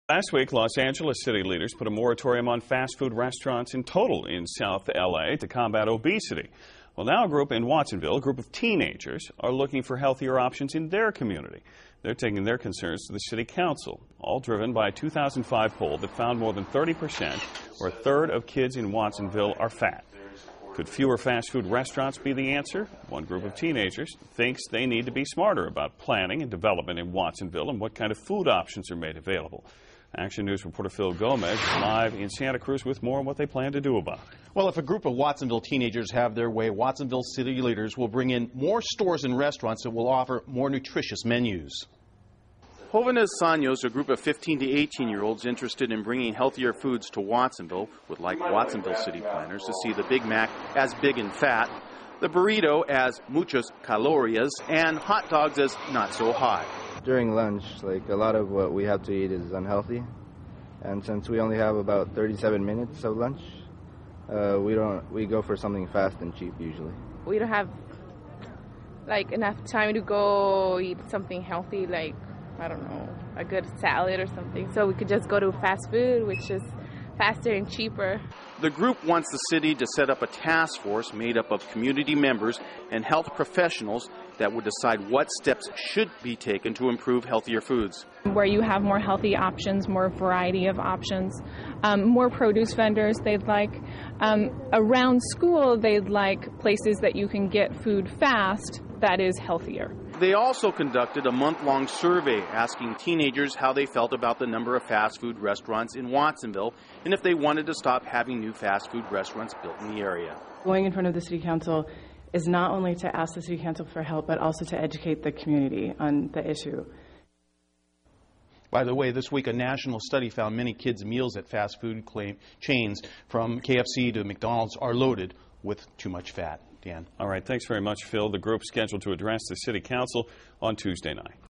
Healthy Food Choices Called For In Watsonville
A group of Watsonville teenagers are calling for healthier food options in their community.